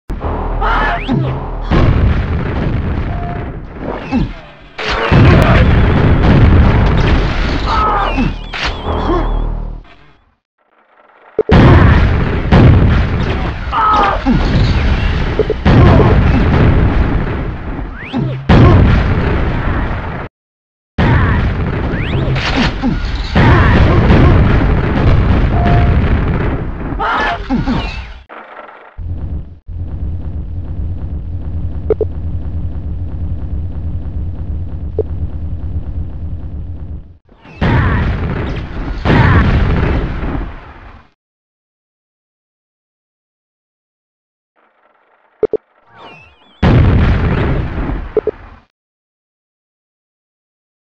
How to Cheat at Rocket Jumping
Why blow yourself up, bouncing off walls, when you can just use rocket boots?

In case it's not clear, the aim here is to not fall into the green goo (which teleports you back to the start). The aim is to get to the black teleporter shown at 0:30 - usually by a series of well timed and placed rocket jumps. It's very hard. So why not just use rocket boots and float on over to the other side?